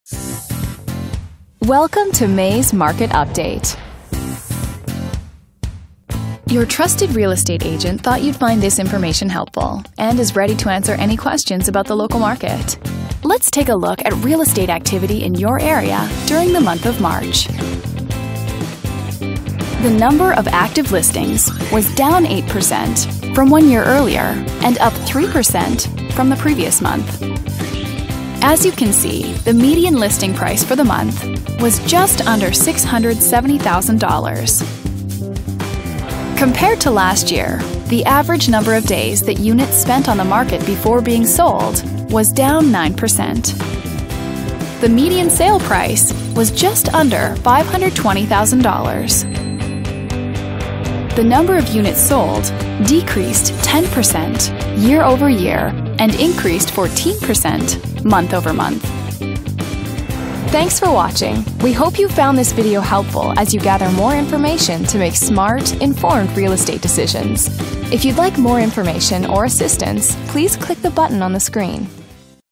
Market Update | May '18 | Nassau County NY | Laffey Real Estate
Laffey RE provides this monthly update of the market in your county on Long Island. [Nassau county]
to search for all the current homes for sale and rentals.
Contact our main office to be paired with your own personal agent.